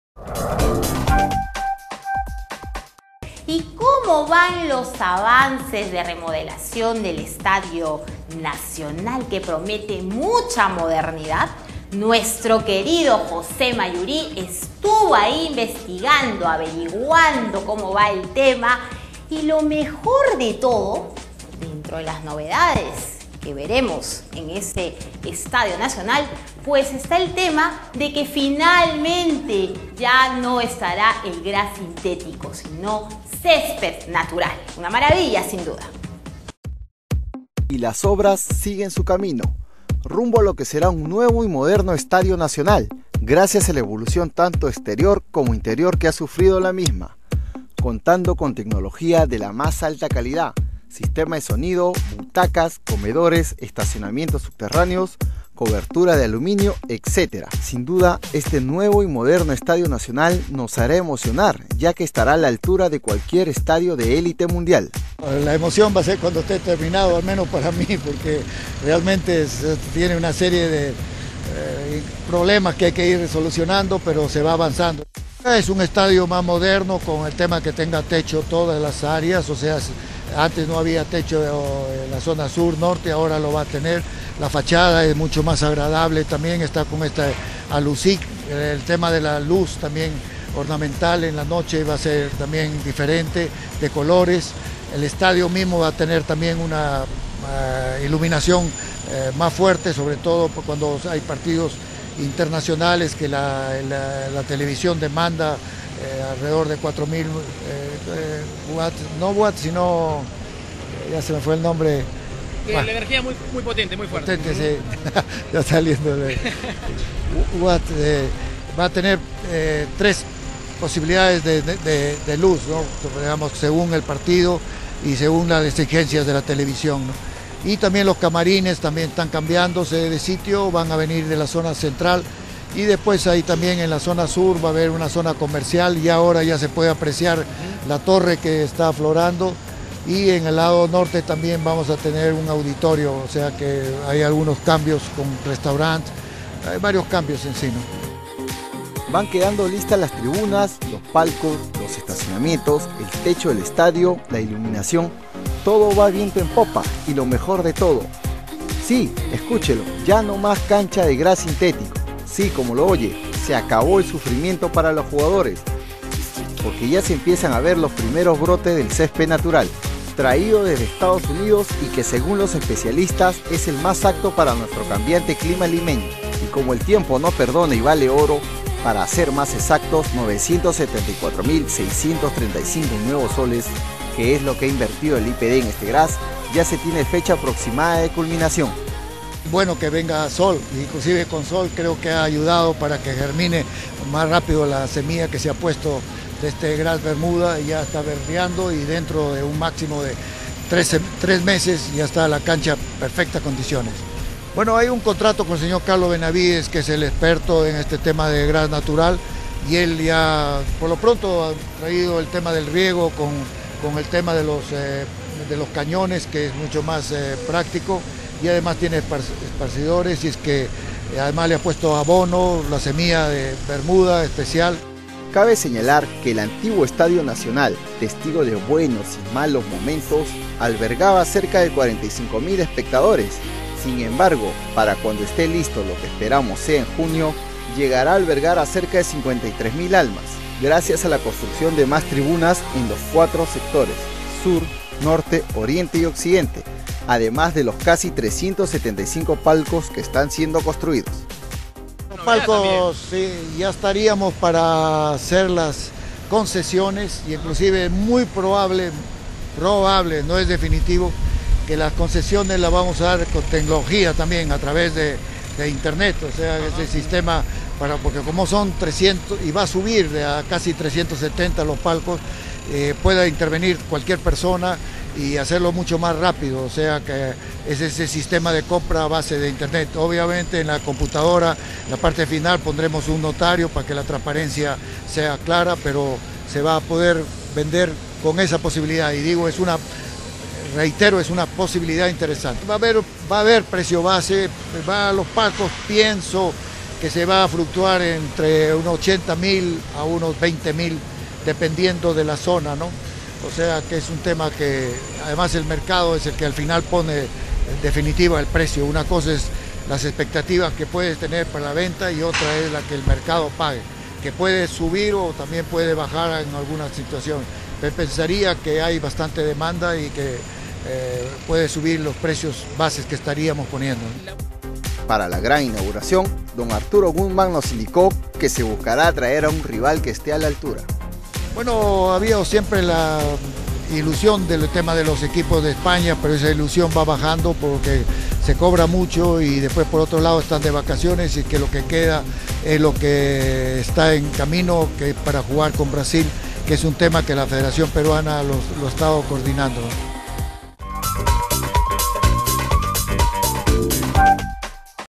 A paso firme sigue la construcción del Estadio Nacional
Las obras siguen su camino rumbo a lo que será un nuevo y moderno Estadio Nacional, gracias a la evolución tanto exterior como interior que ha sufrido la misma, contando con: Tecnología de la más alta calidad, sistema de sonido, butacas, comedores, estacionamiento subterráneo, cobertura de aluminio, etc. Sin duda este nuevo y moderno Estadio Nacional estará a la altura de cualquier reciento de nivel mundial.
Van quedando listas las tribunas, los palcos, los estacionamientos, el techo del estadio, la iluminación todo sigue viento en popa y lo mejor de todo ya no mas cancha de grass sintético, si como lo oye se acabo el sufrimiento para los jugadores, porque  ya se empiezan a ver los primero brotes del césped natural "Bermuda Americana Mejorado" traído desde Estados Unidos y que, según los especialistas, es el más apto para nuestro cambiante clima limeño.